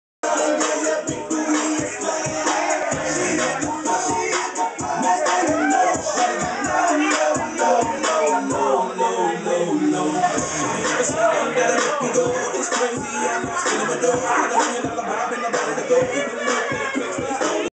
The Lawnmower
Josh's lawnmower dance move